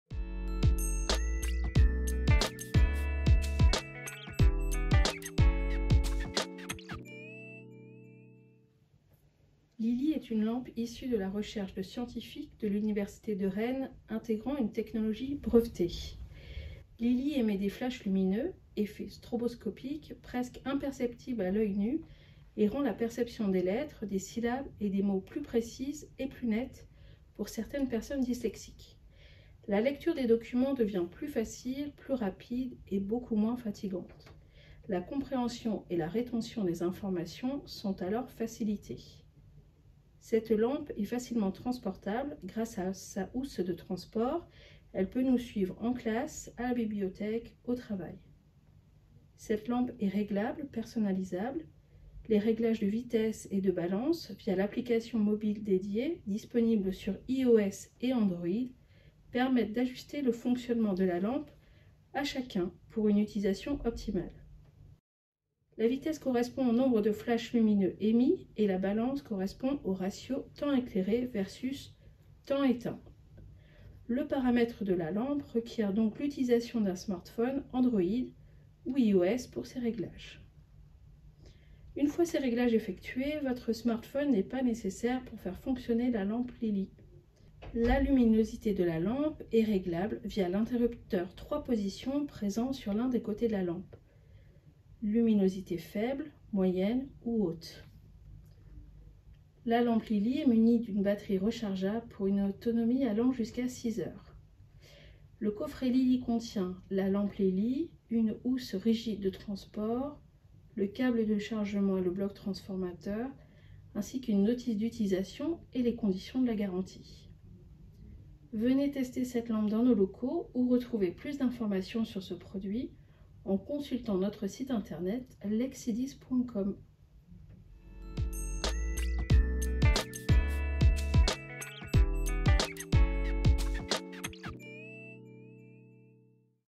Lampe Lili
LILI EST UNE LAMPE QUI FACILITE LA LECTURE AUX PERSONNES DYSLEXIQUES
Basée sur les recherches scientifiques (Université de Rennes), la technologie intégrée de la lampe Lili émet des flashs lumineux, presque imperceptibles à l’œil nu, et rend la perception des lettres, des syllabes et des mots plus précise et plus nette pour de nombreuses personnes dyslexiques.
La lecture de tout document devient plus facile, plus rapide et beaucoup moins fatigante.
La compréhension et la rétention des informations sont alors facilitées.